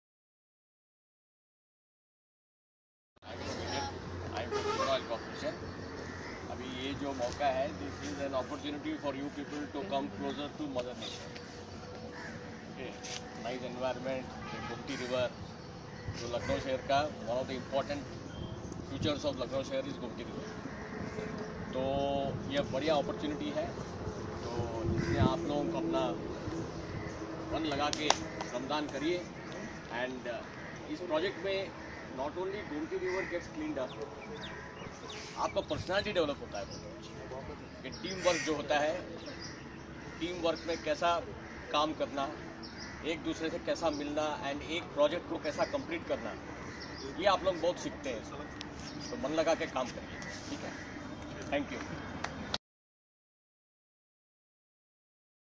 22 Dec 2017- Address by IOCL GM Bhushan ji in Prithvi’s “Gomti Gaatha” Gyanshaala program.
Importance of cleanliness and how to developing your personality through cleanliness campaign address by IOCL GM Bhushan ji at Prithvi’s “Gomti Gaatha” nature workshop at Ghaila pul.